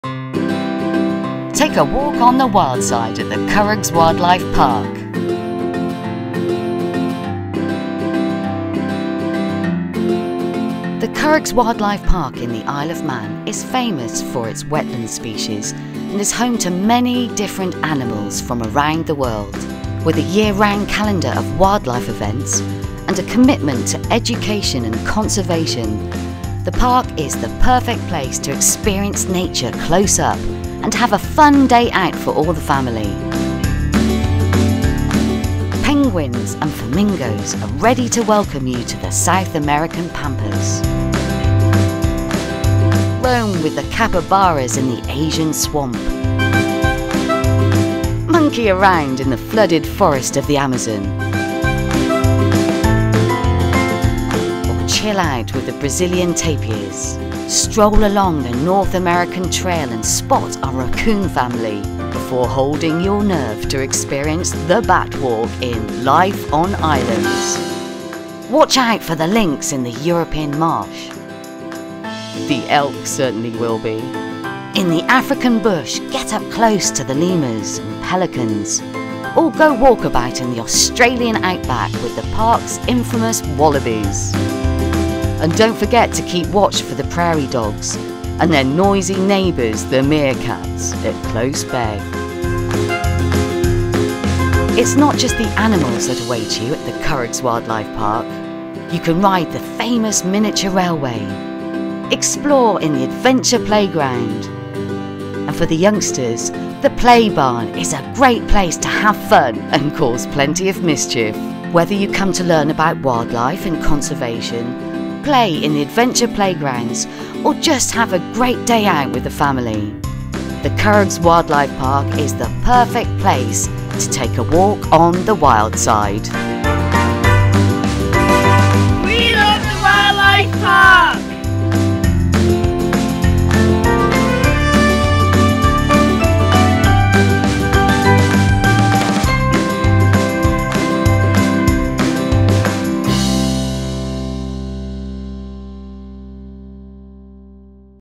Come to the Curraghs Wildlife Park
The Curraghs Wildlife Park in the Isle of Man is famous for its wetland species and is home to many different animals from around the world. With a year round calendar of wildlife events and a commitment to education and conservation, the Park is the perfect place to experience nature close up and have a fun day out for all the family.

Video produced by Reel Vision